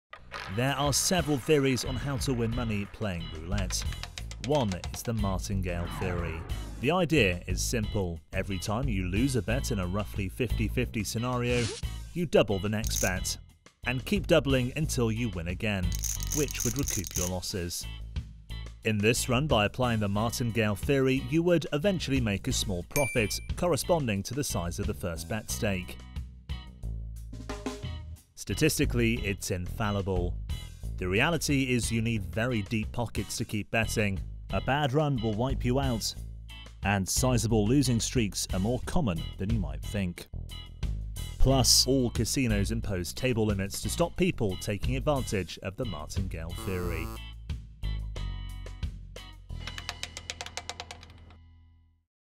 How to Use Martingale Strategy in Roulette
If you would like to begin with the most simple strategy of those available, we recommend that you watch this video and start with the Martingale system: If you want to find more strategies about how to play roulette online go